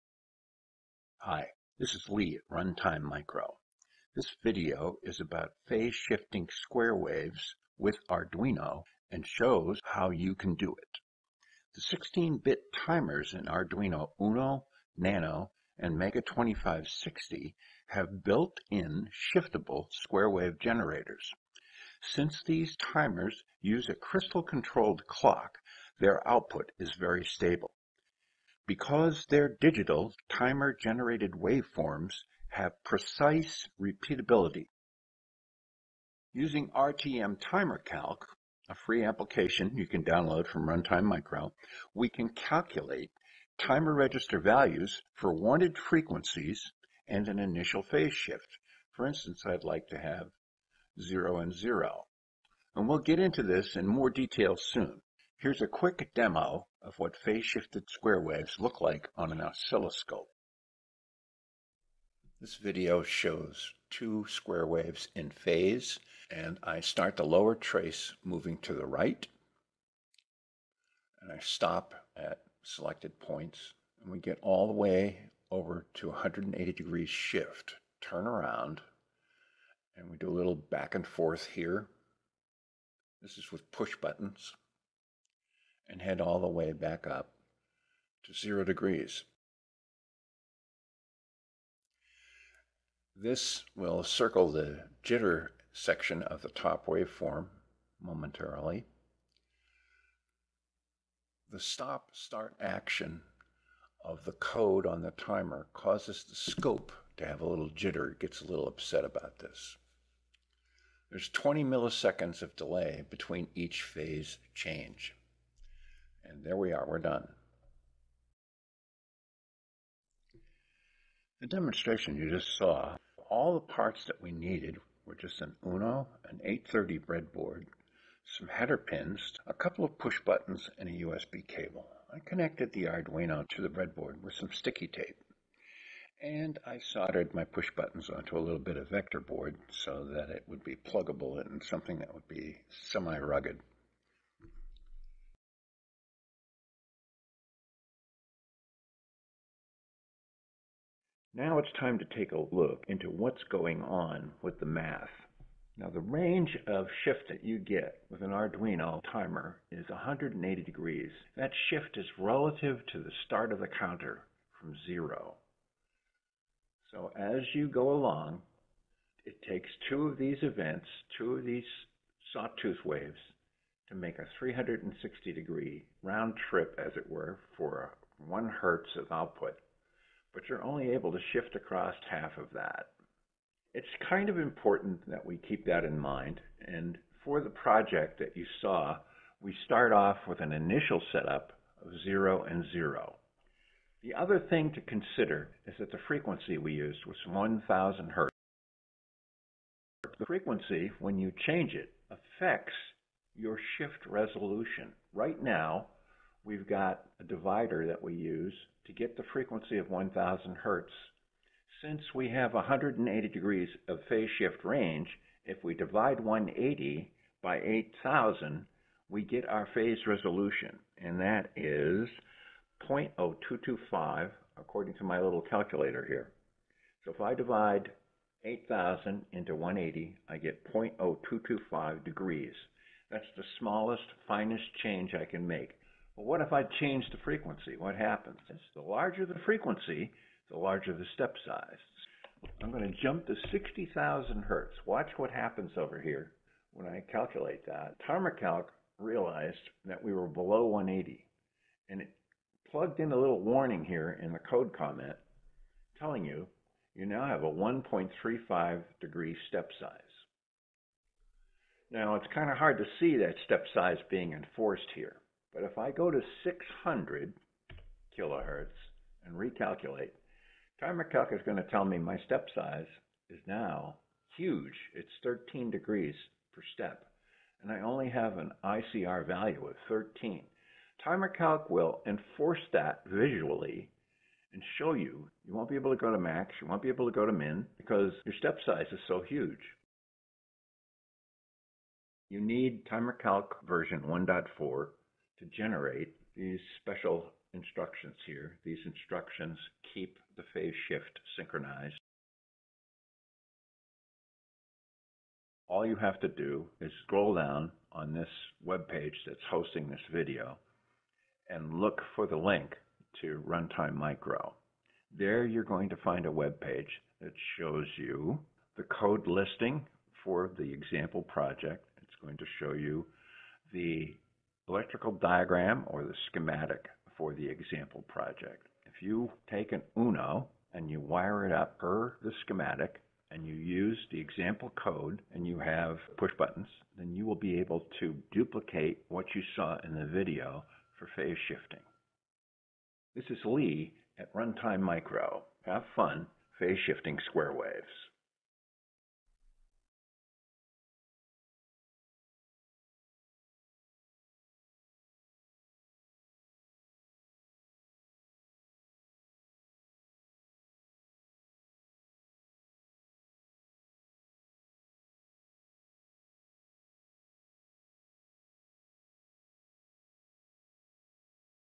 Phase Shift Square Waves
Demonstrates Arduino Uno smoothly phase-shifting square-waves across a 180 degree range. Reveals how to easily generate needed Timer C++ code.

ERRATA: Info-callouts showing the number of angles are incorrect (each should be smaller by 1). Another error is dividing 180 by 8000, it should be180 / 7999. I keep forgetting the ICR = value -1 notation is actually real. This is because we can't use Zero for an OCR value.  My bad  : /